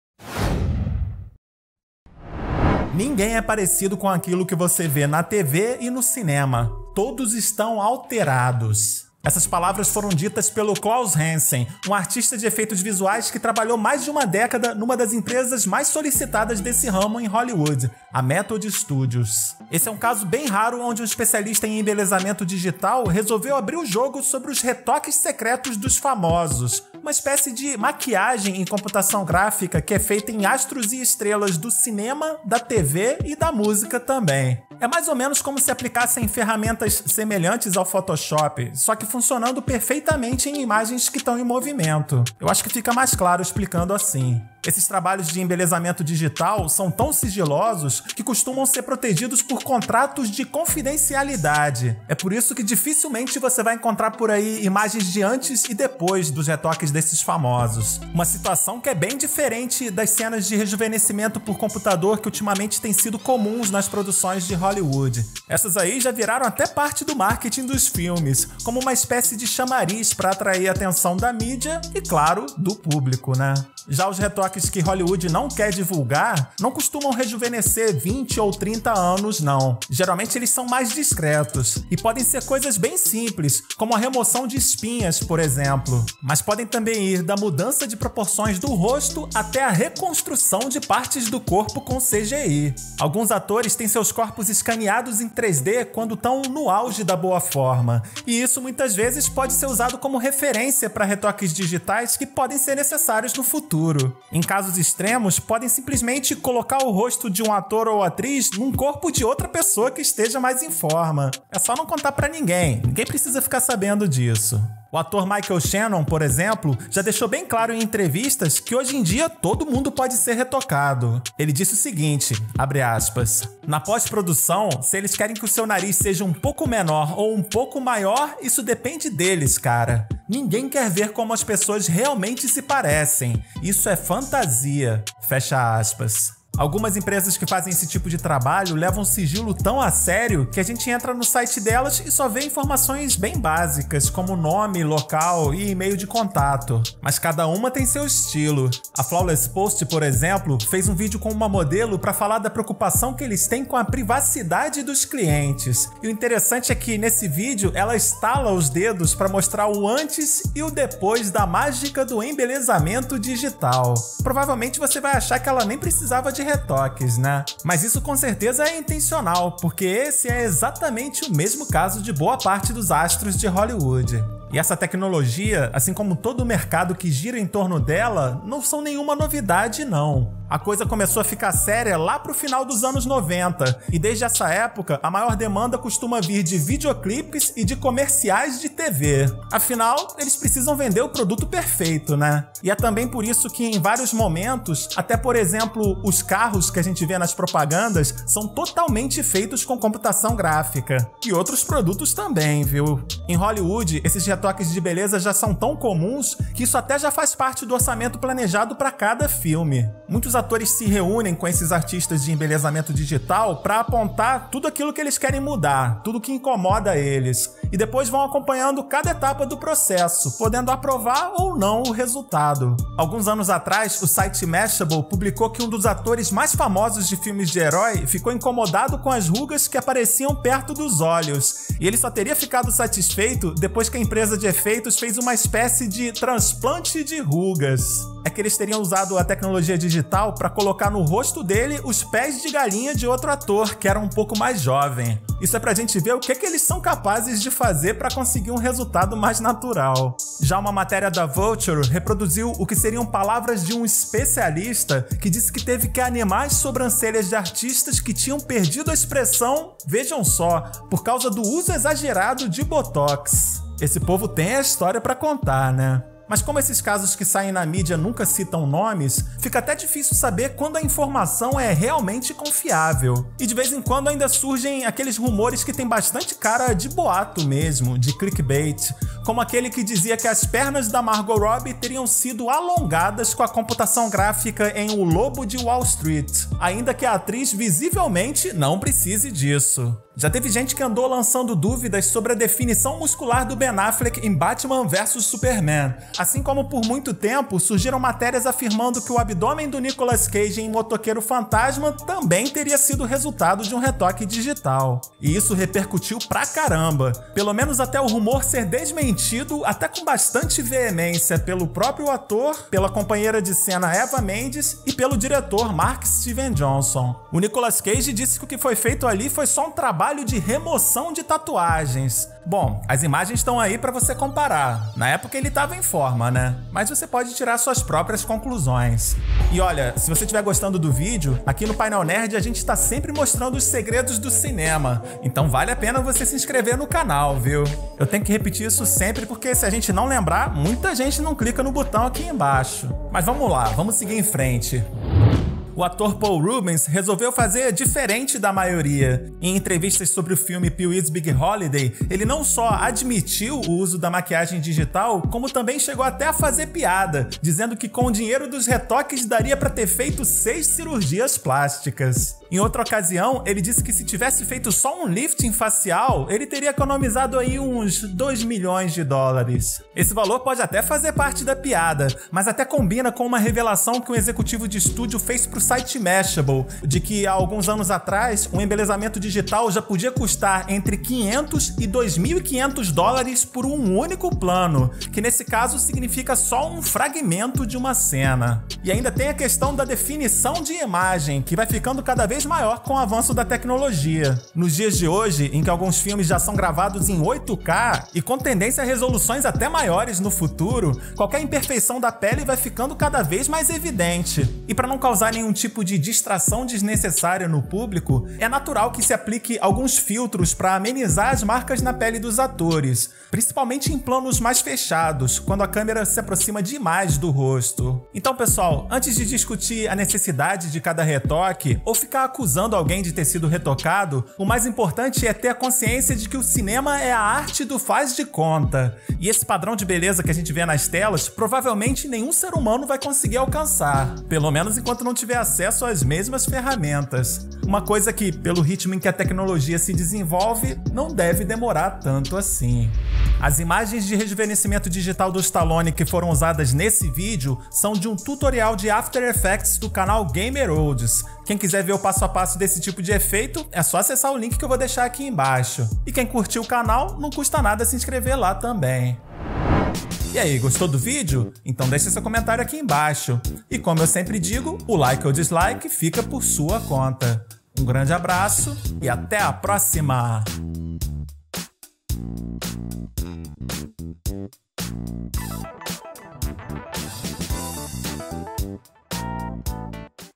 Um dos MAIORES SEGREDOS de Hollywood! A MAQUIAGEM DIGITAL que NINGUÉM quer revelar!
"NINGUÉM se parece com o que você vê na TV e no cinema - TODO MUNDO está alterado".
Essa frase é de um especialista em efeitos de embelezamento digital com mais de uma década de experiência em Hollywood. Nesse vídeo, você vai saber mais sobre os retoques que são feitos nos astros por computação gráfica, mas Hollywood nunca vai revelar pra você!